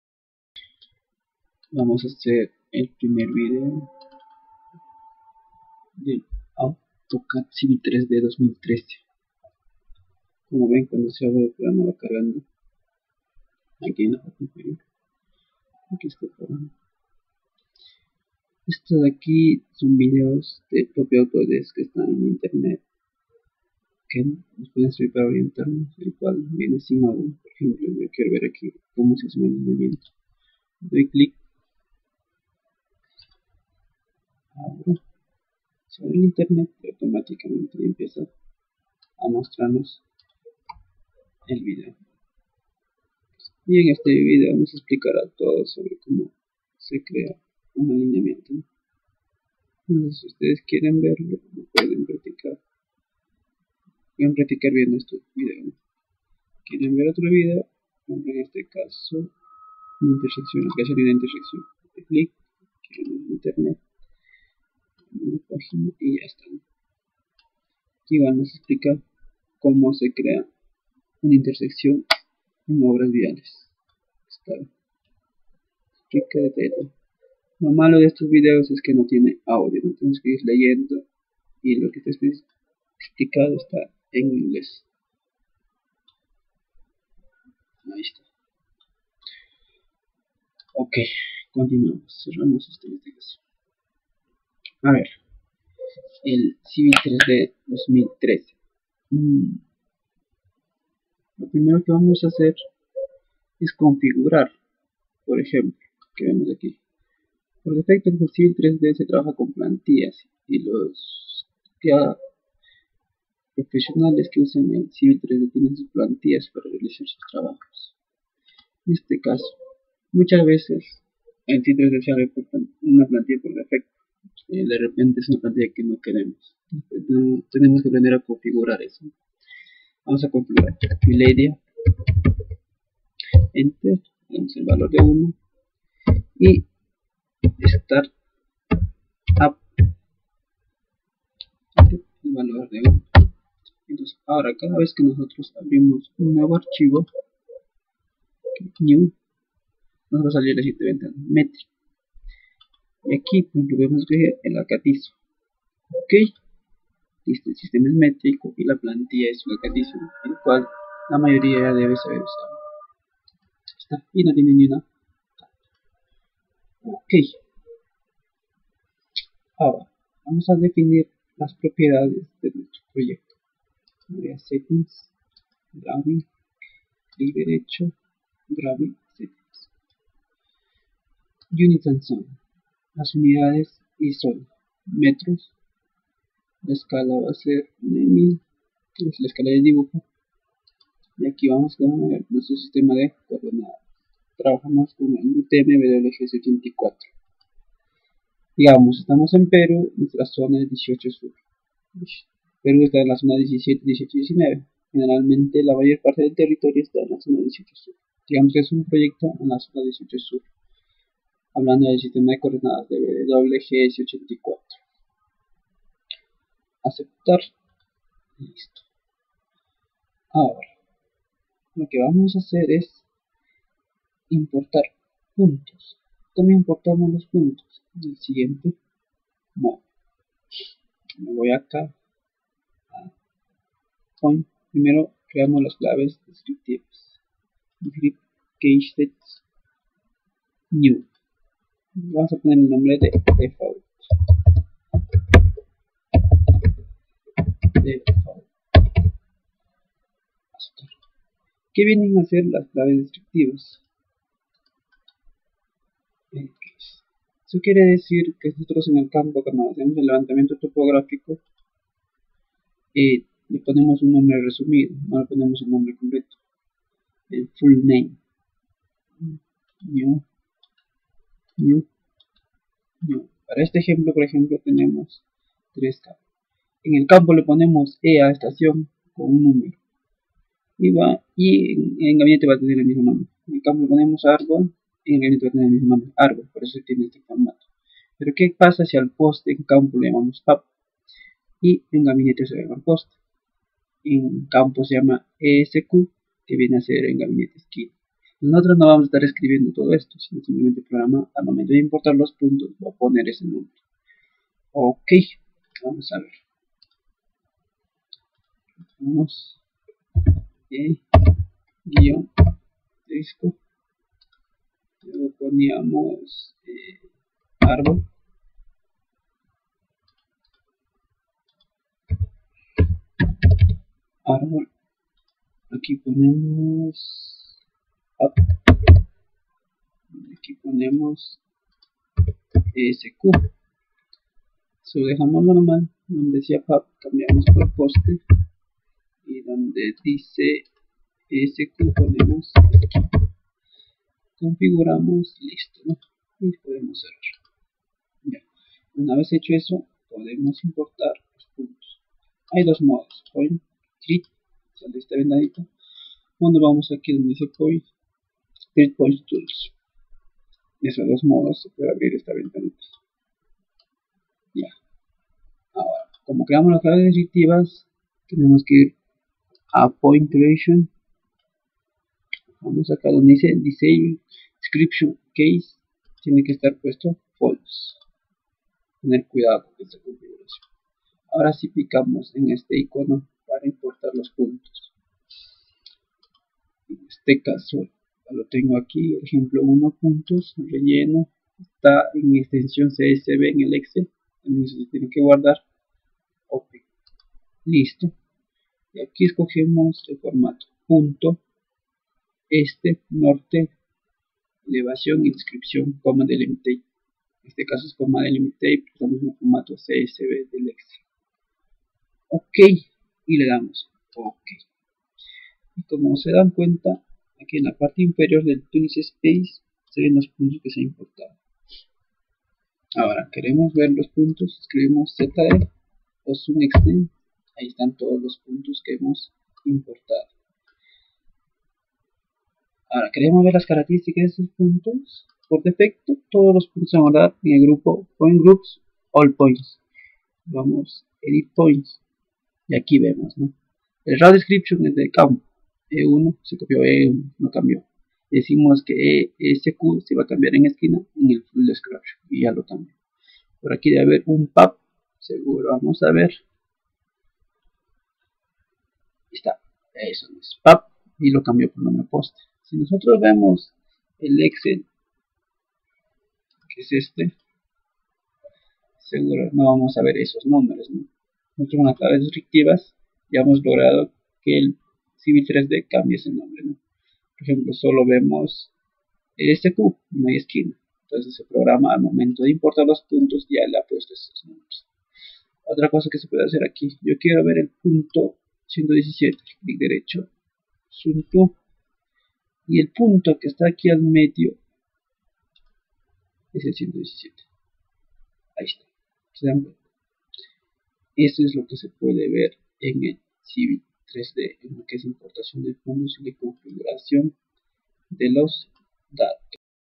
CIVIL 3D 2013 - (1/10) Importación de Puntos
En este primer vídeo se explica sobre la configuración del AutocaCAD Civil 3D, y también sobre la importación de puntos teniendo en cuenta el Uso de claves descriptivas.
Las claves descriptivas nos sirven para cambiar los nombres que se le da a los puntos en los Levantamientos Topograficos por un nombre mas tecnico en el Gabinete